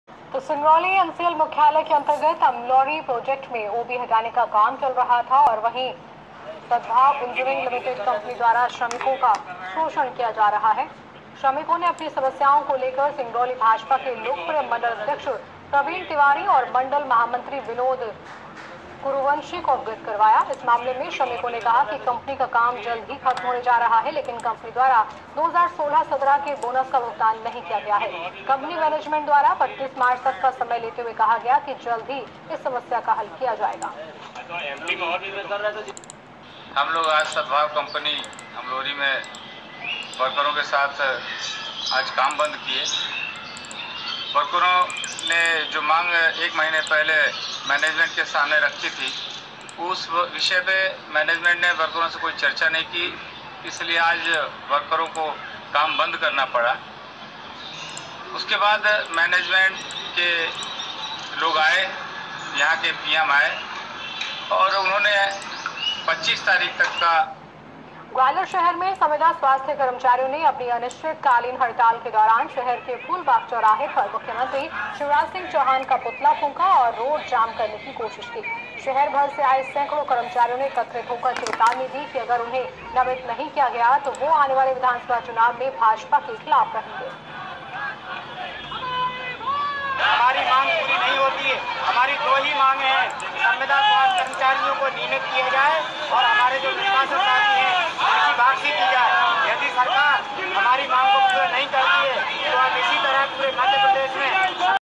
SINGROULI
jk24x7 news चैनल पर देखिये * सिंगरौली में श्रमिको ने की कंपनी से बोनस की मांग